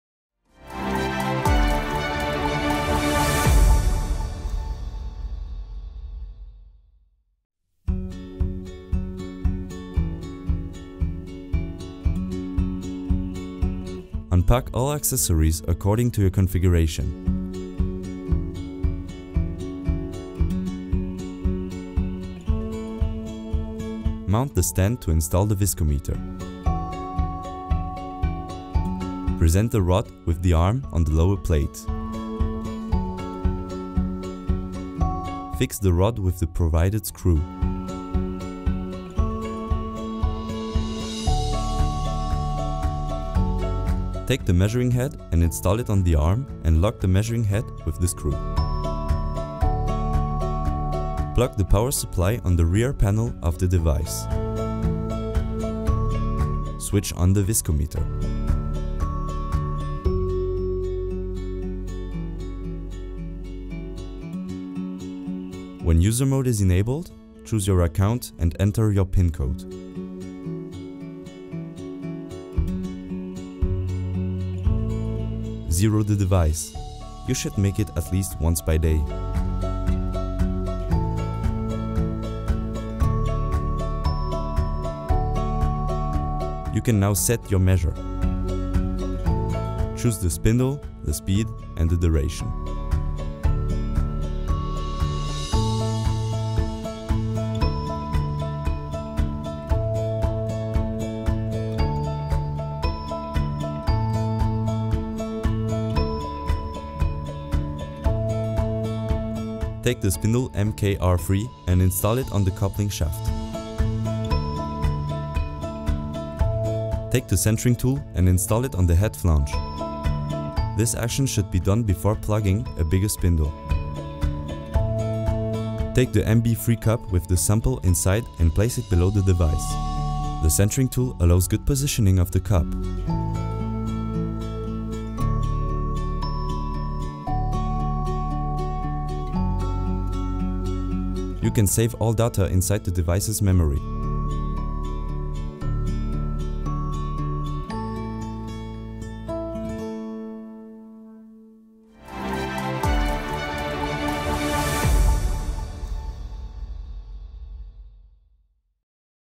RM100 PLUS and MSR3
Installation and measurement with viscometer RM100 PLUS and measuring system MS-R3